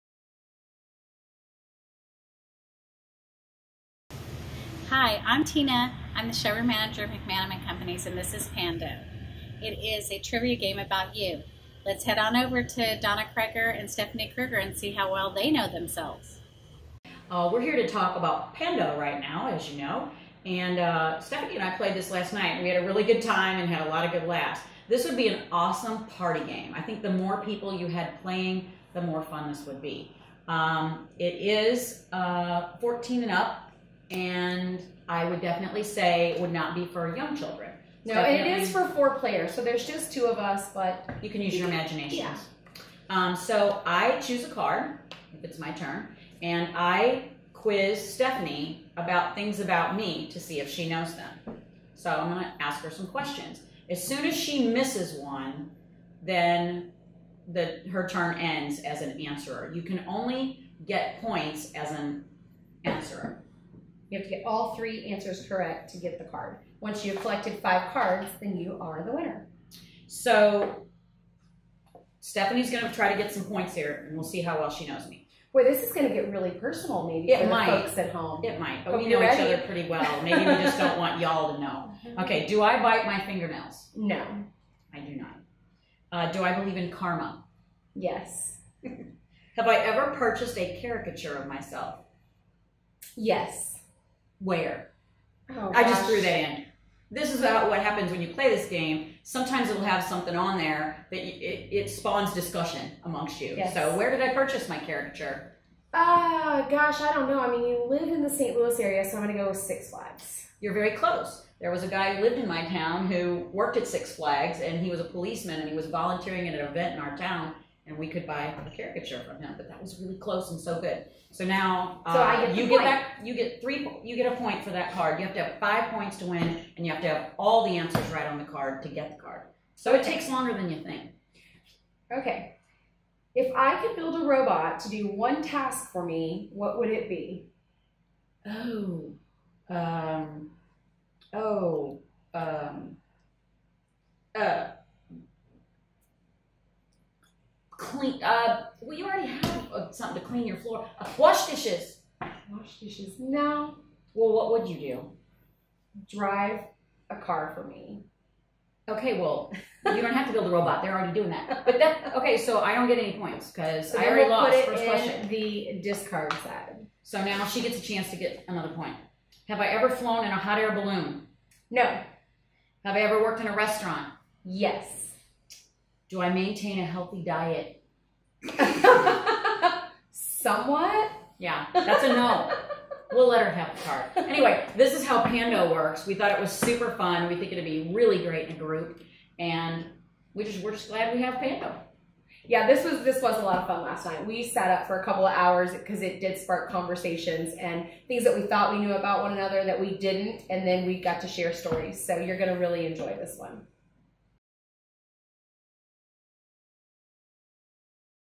Pando
Pando, The Trivia Game About Us, is the get to know you/personality game for
ages 14+.

"Pando- June Virtual Show 2020
$11 per unit with FOB Boise, Idaho. Valid June 22nd – June 30th, 2020"